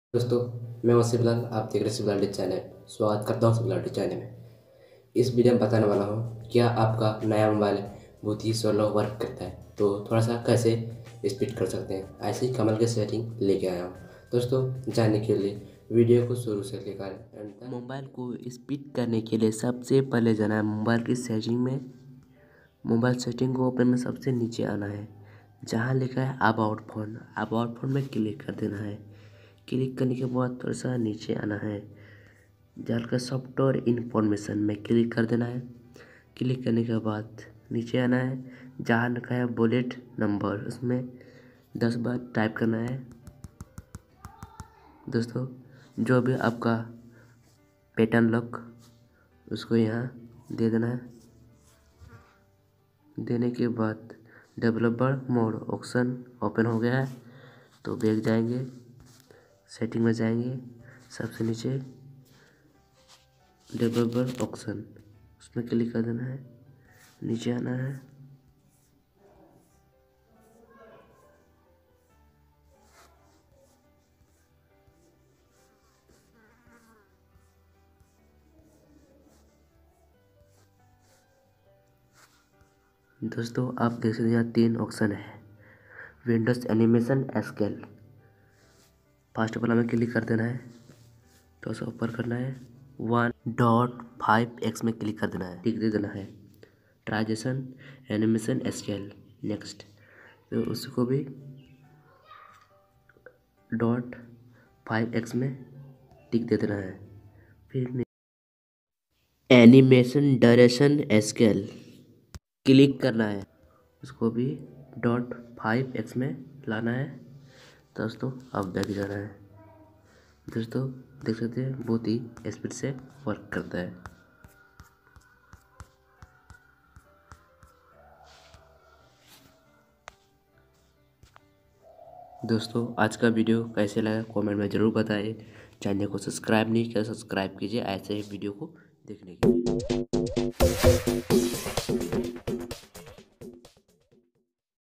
How To Amazing Setting Mobile speed /आप का मोबाइल को Speed करेंगा एक Setting से
👉Aaj Is Video Me Kamal Ki Setting Jise aapka mobile ko speed Karthika